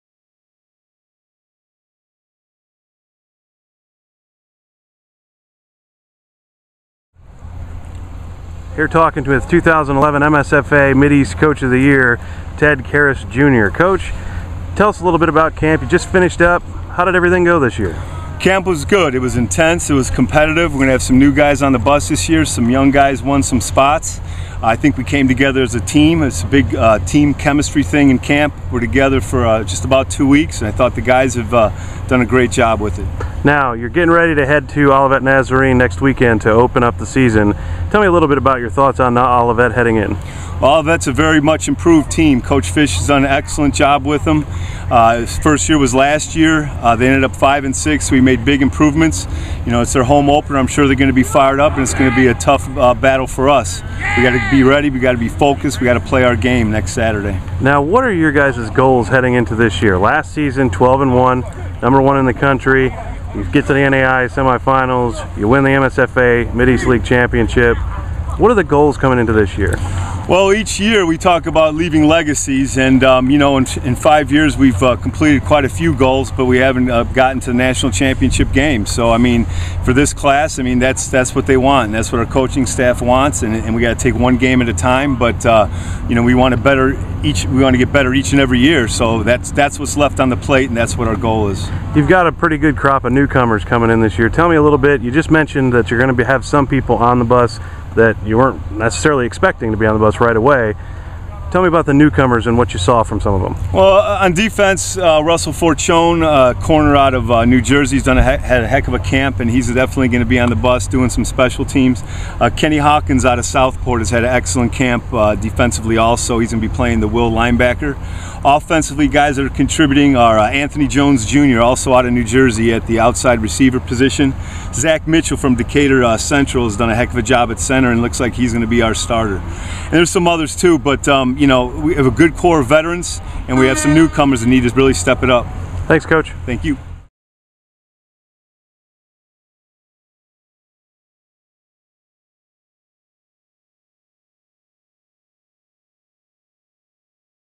Camp Notes 2012 - Head Coach Ted Karras Jr.
Head coach Ted Karras Jr. talks with us about camp, the expectations for 2012 and some surprises heading into the season opener next weekend.